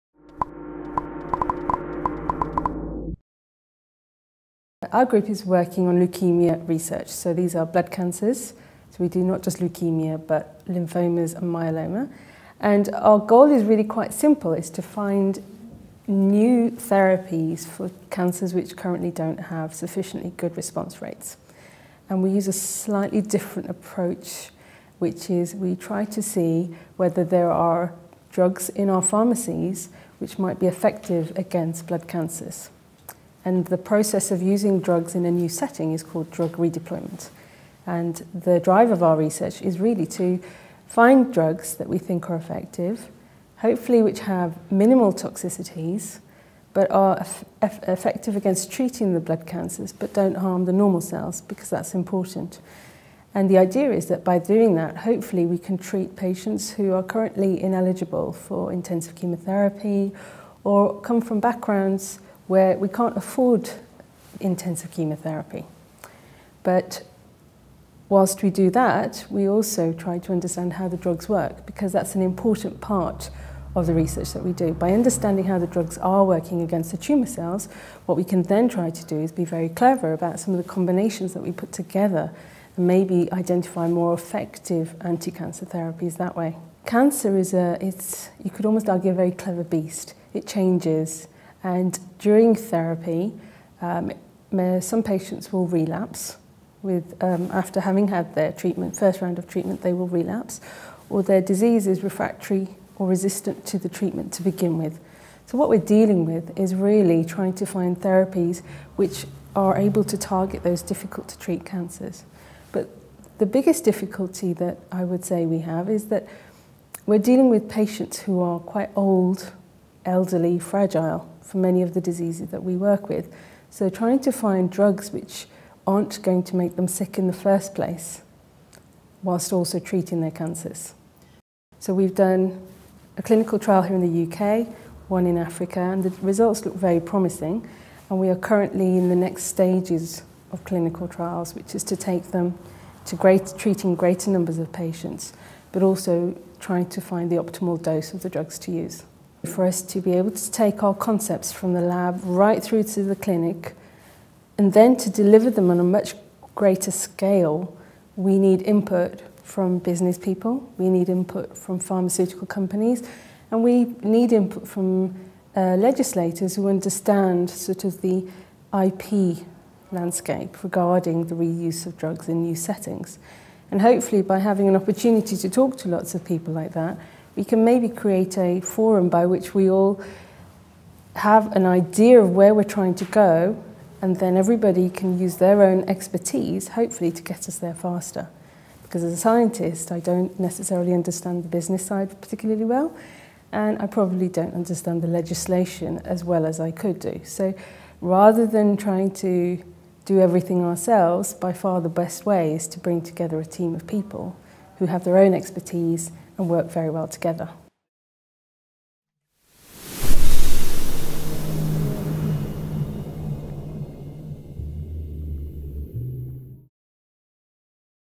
Drug redeployment may help beat cancer, says biologist
Drug redeployment strategies will lead to the discovery of novel therapies which can beat haematological cancers, a senior researcher at the University of Birmingham has said.

Using off-patent drugs to provide affordable therapies, Dr Farhat Khanim's research aims to find drugs which have minimal toxicities and are effective against treating the blood cancers without harming normal cells.